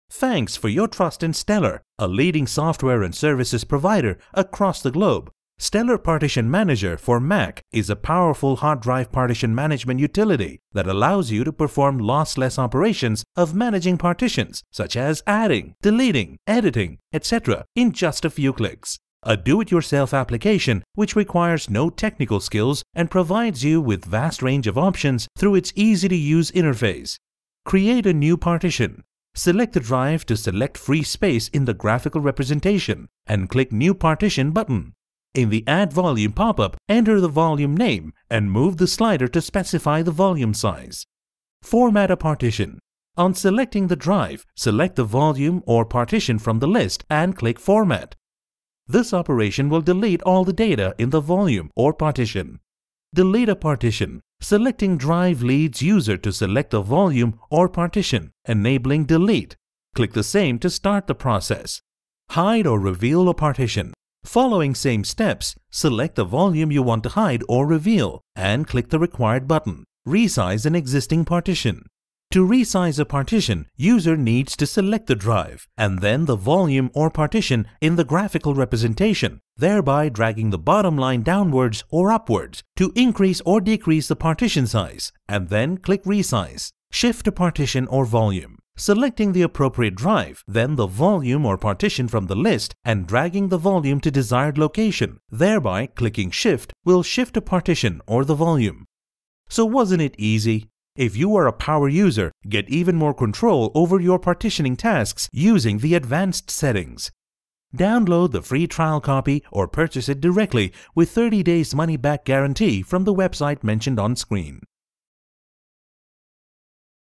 Howto: Resize Mac-Partition Mac hard drive
Partition Manager can help you create, format, delete, hide & reveal, shift or resize the selected partitions/volumes on your Mac hard drive. Add more space to your Mac by using Mac Partition Manager